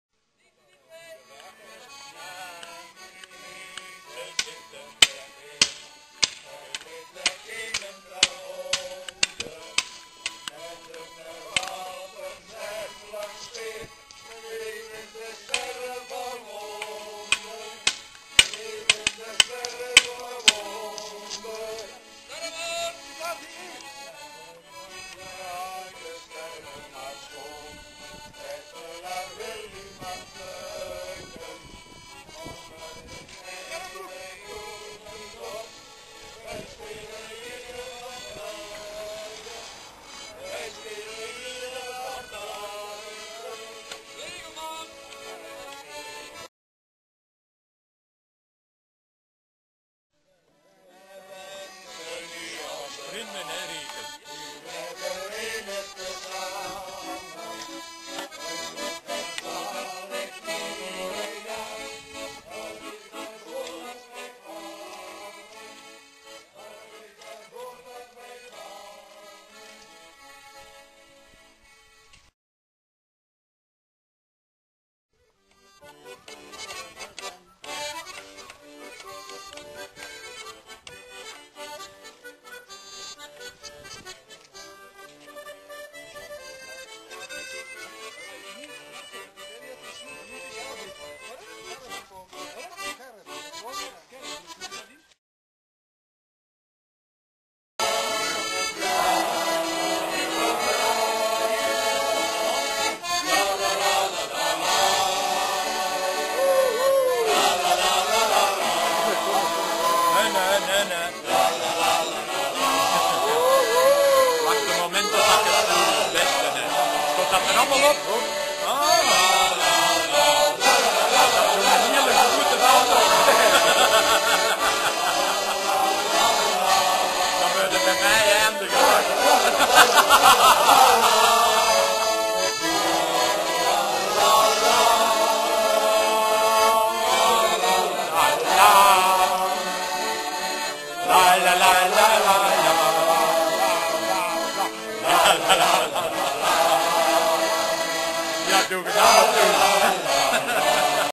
berenstoet wichelen 2
Elk jaar gaat in Wichelen de Berenstoet uit een oud folkloristisch gebeuren